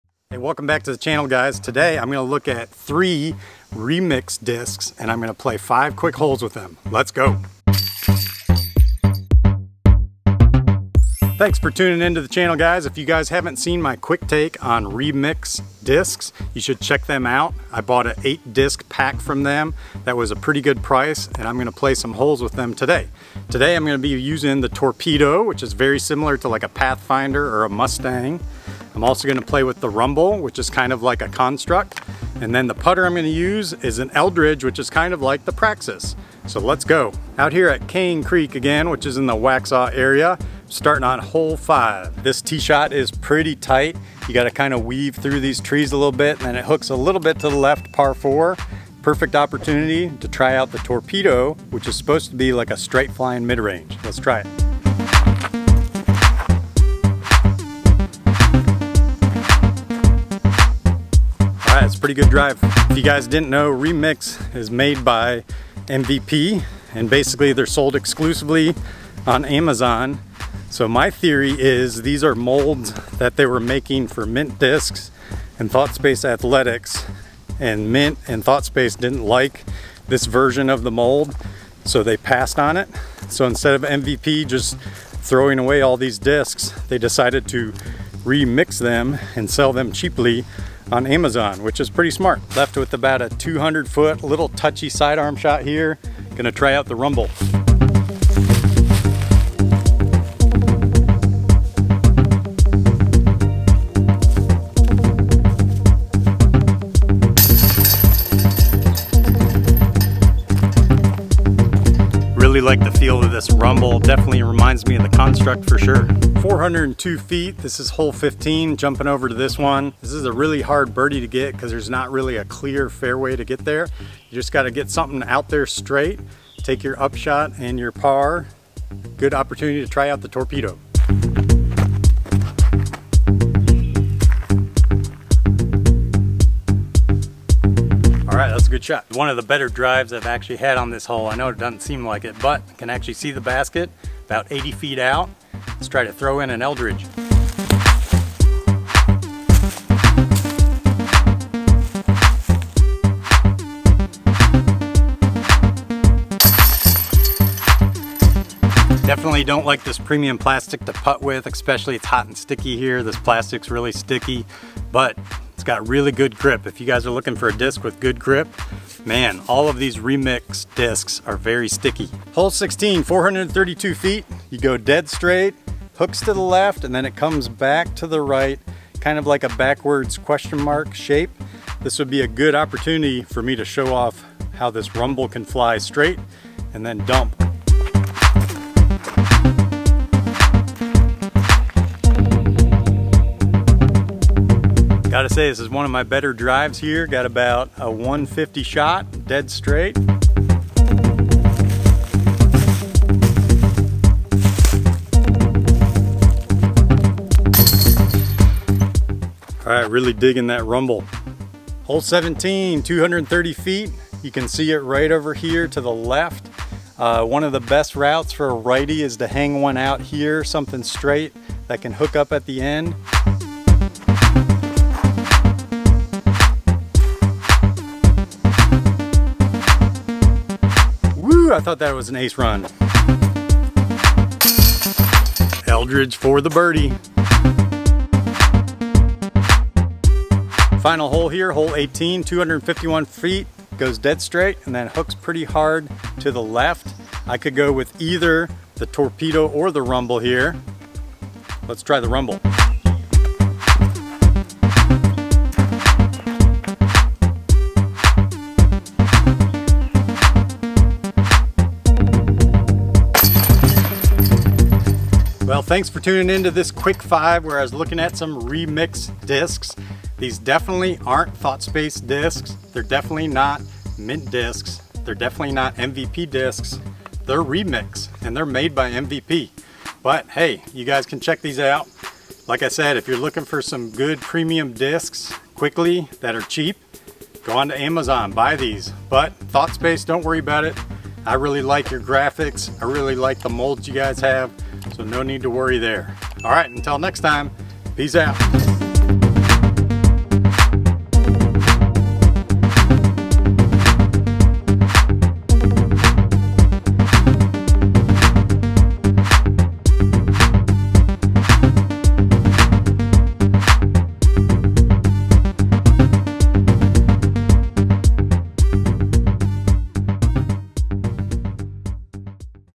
DISC REVIEW: Torpedo, Eldritch and Rumble (Remix Disc Golf)
Jeremy does a QUICK TAKE on the REMIX DISCS Torpedo, Eldritch and Rumble.